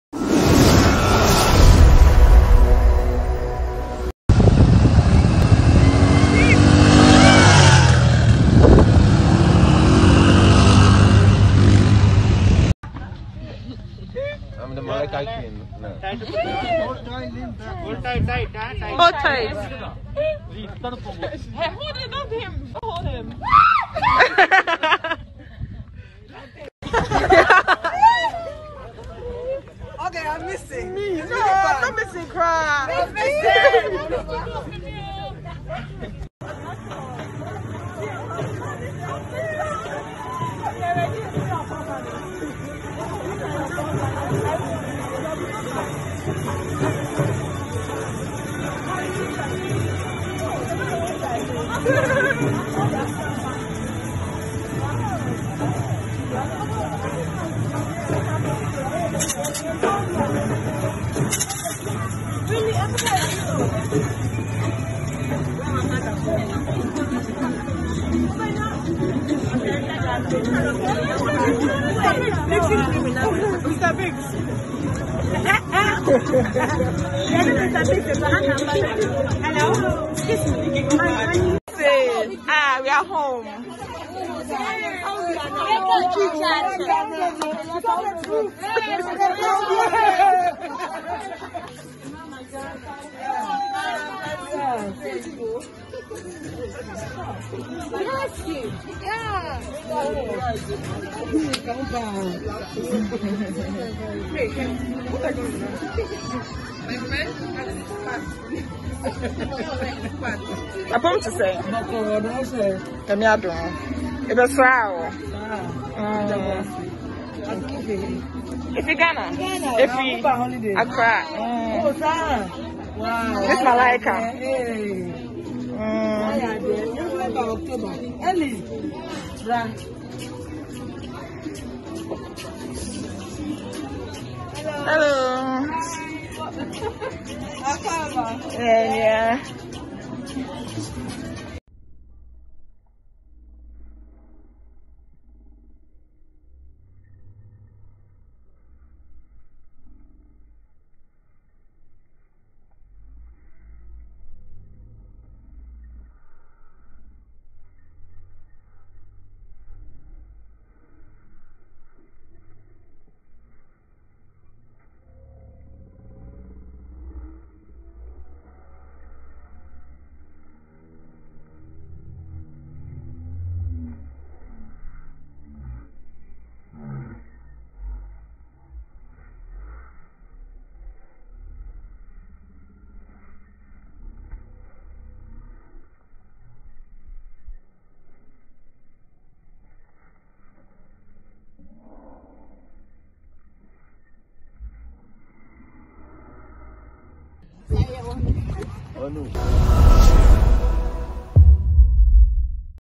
Miss Malaika finalists chilling in Dubai
The finalists of miss malaika tour Kenya and Dubai